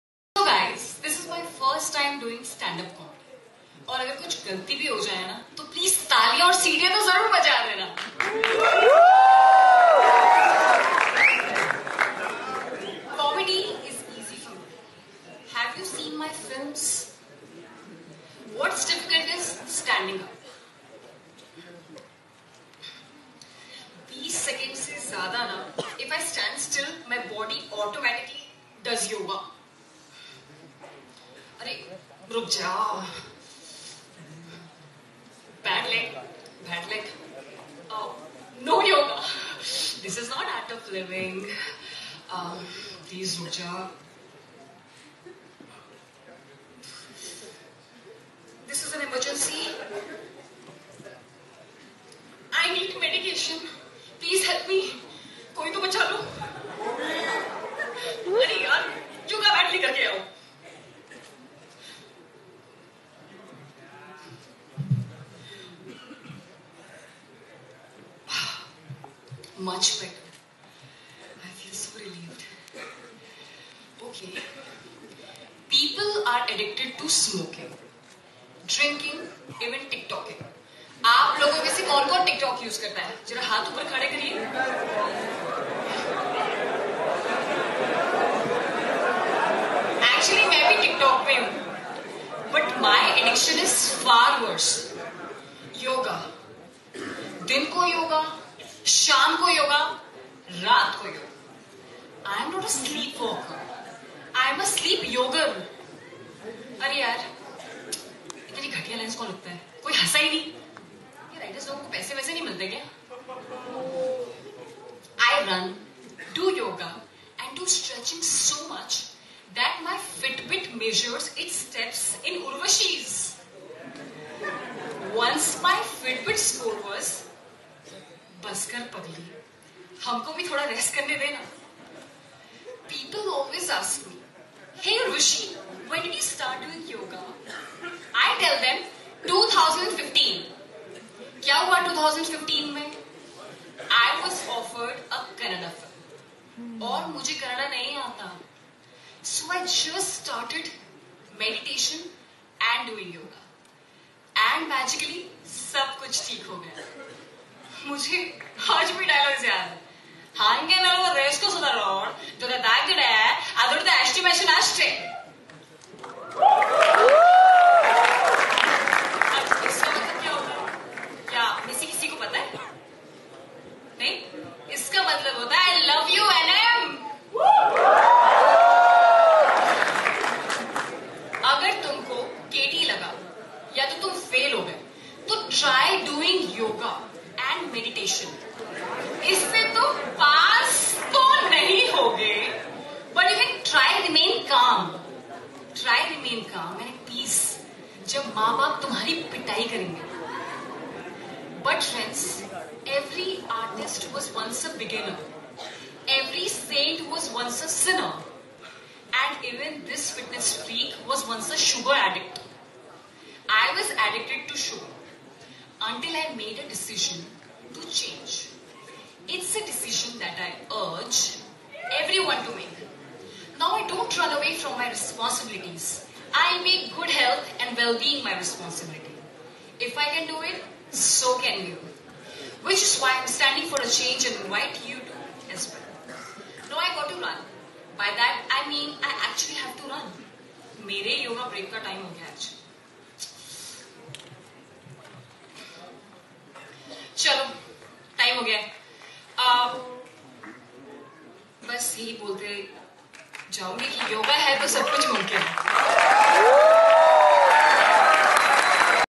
URVASHI RAUTELA DOING STAND UP COMEDY AT COMICS FOR CHANGE STAGE
movie india is a stage where you find every information related to cinema in india.